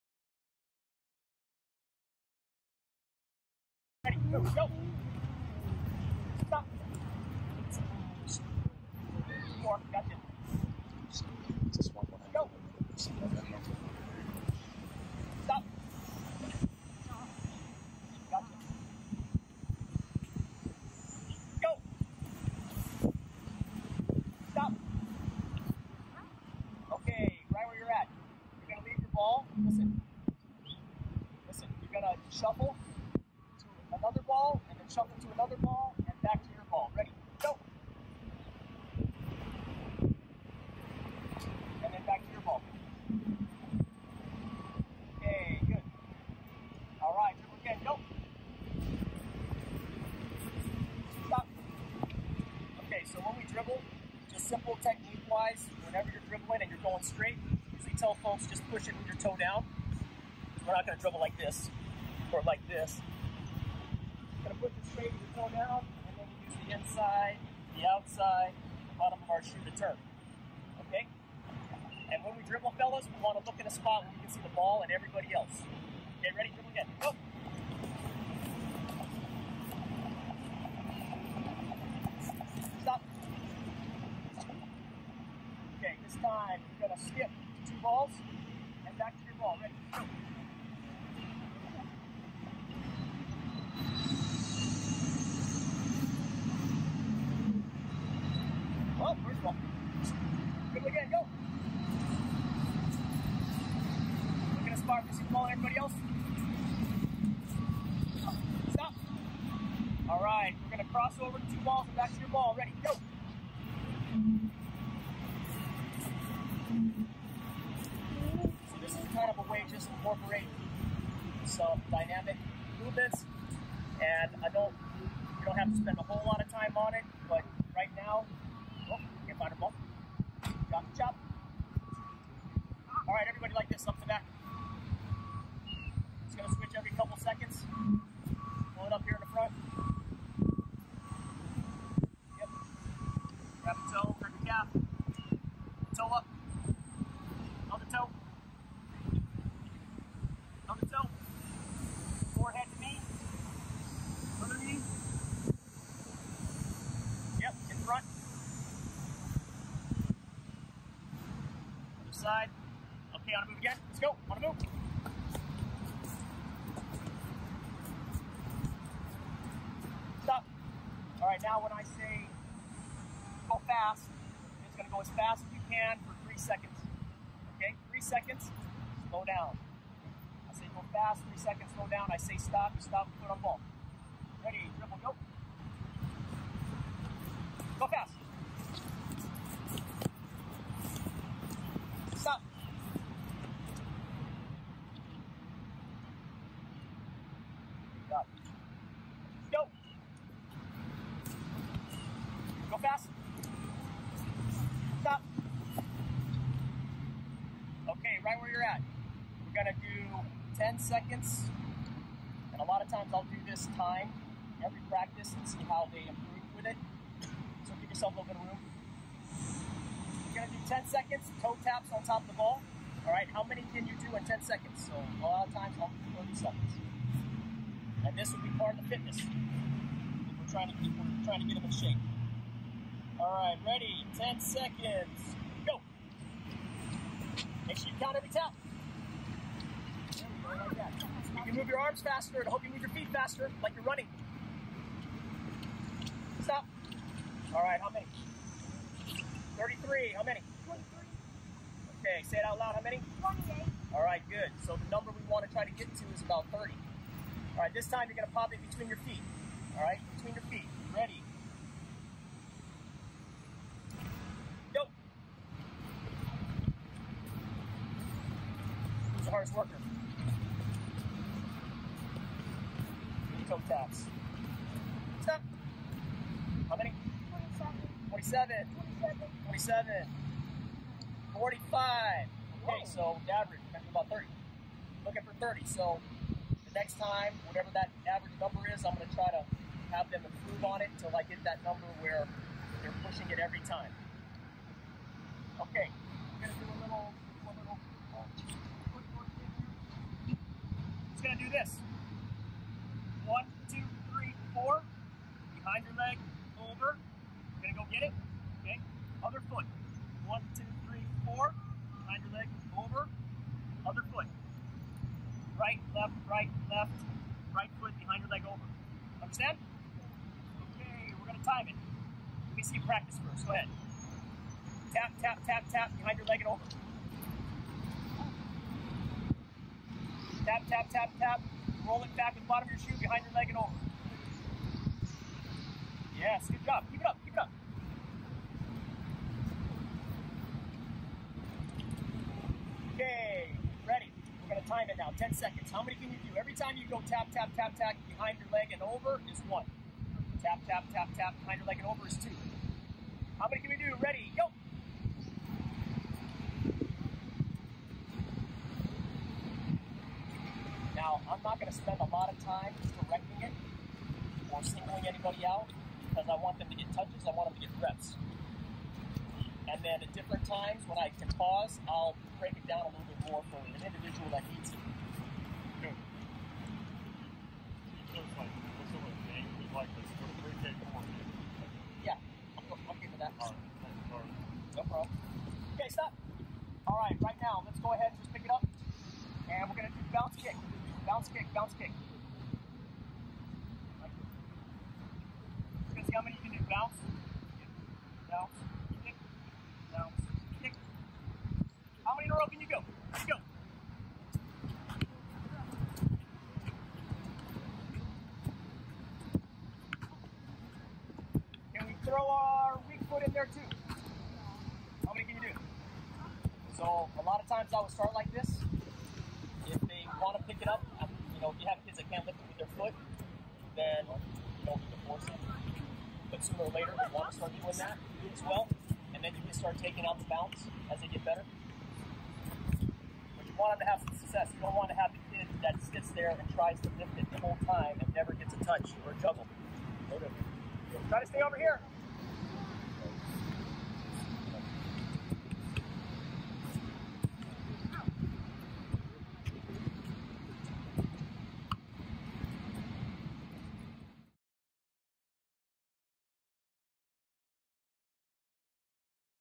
Coaching Corner Training Session Ideas #1- Stop & Go and Timed Skills
Executive Director of BRSC, Louie Smotherman, covers some simple training exercises coaches can use for team practices. This video introduces the Stop & Go game and a Timed Skills drill.